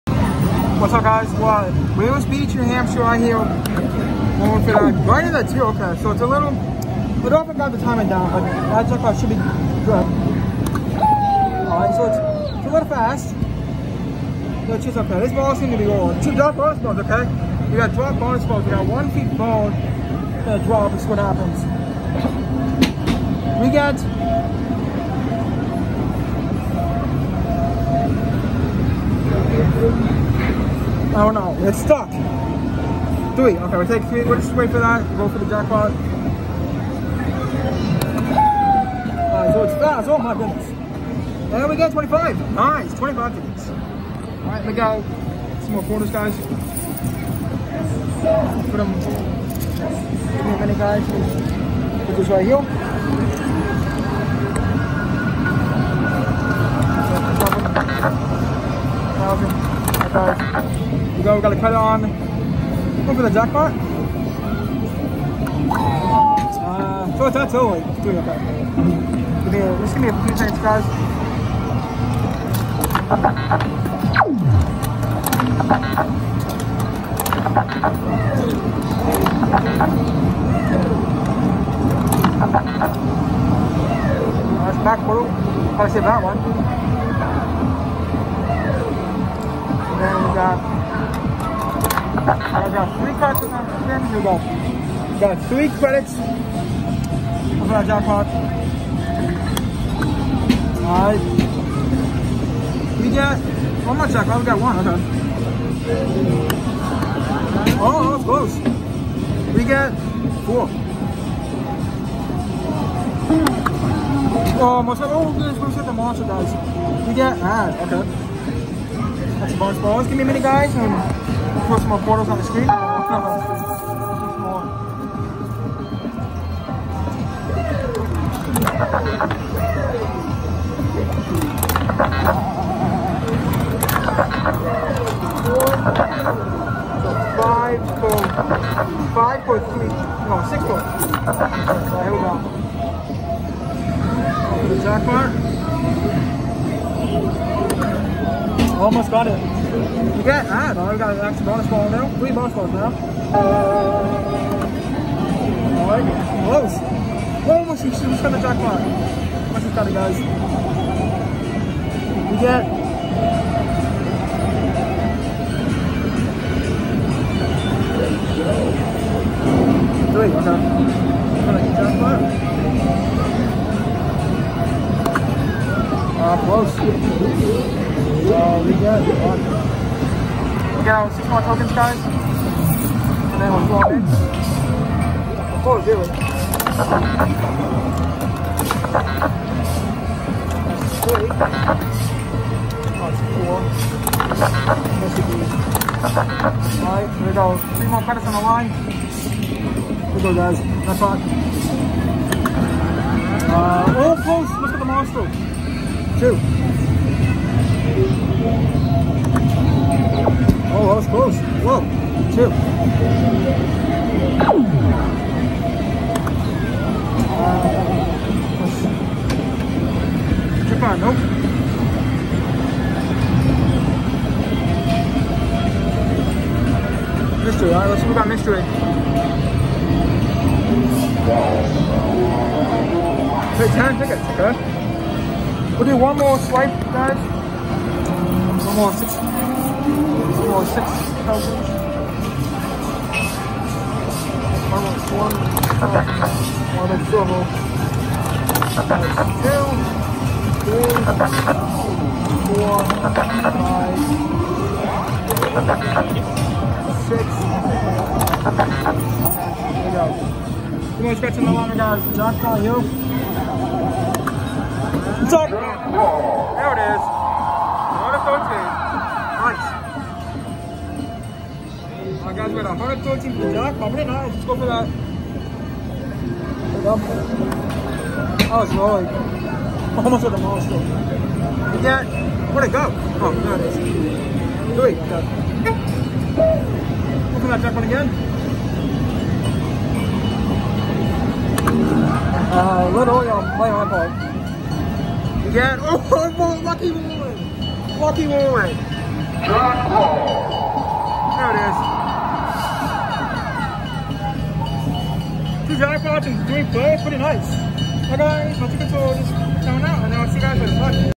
Late night monster drop action from Weirs Beach New Hampshire!
Monster drop action for y’all! Update will be coming on why the last NHL 21 game was deleted yesterday and don’t worry it’s not for anything bad. I’ll re film it soon and have a great night everyone :)